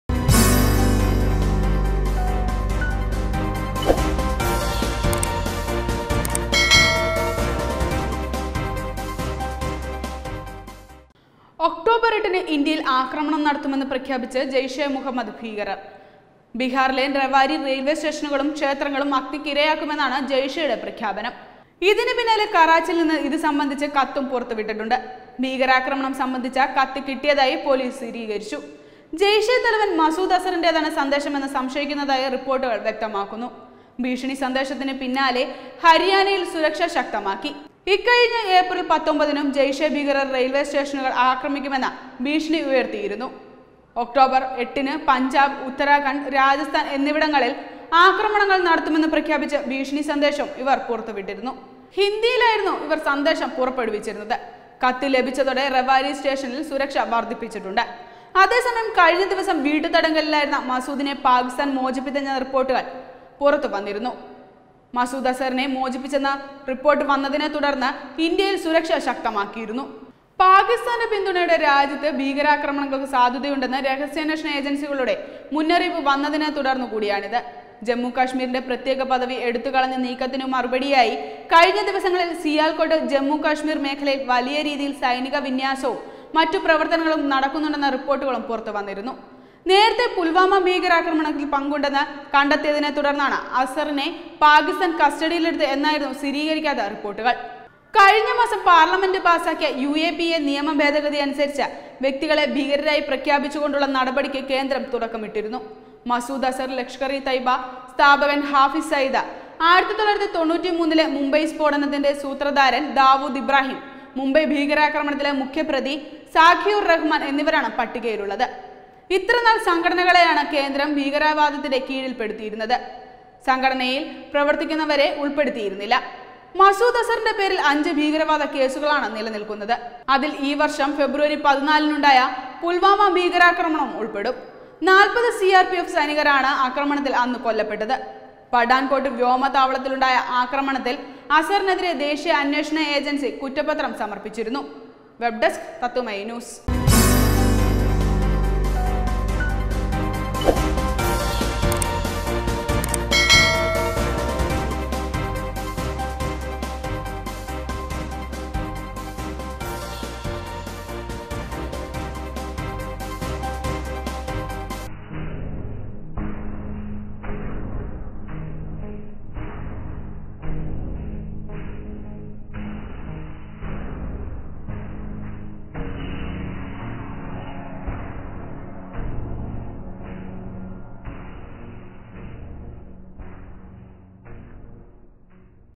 ഇമ്രാന്‍ഖാനെ ചങ്ങലക്കിട്ട് ഭീകരര്‍; ഇന്ത്യയ്ക്ക് നേരെ ഭീകരാക്രമണ ഭീഷണി
ഒക്ടോബര്‍ എട്ടിന് ഇന്ത്യയില്‍ ആക്രമണം നടത്തുമെന്ന് പ്രഖ്യാപിച്ച് ജെയ്‌ഷെ മുഹമ്മദ് ഭീകരര്‍.ബിഹാറിലെ റെവാരി റയില്‍വേസ്റ്റേഷനും ക്ഷേത്രങ്ങളും അഗ്നിക്കിരയാക്കുമെന്നാണ് ജയ്‌ഷെയുടെ പ്രഖ്യാപനം. ഇതിന് പിന്നാലെ കറാച്ചിയില്‍ നിന്ന് ഇത് സംബന്ധിച്ച്  കത്തും പുറത്തുവിട്ടിട്ടുണ്ട്.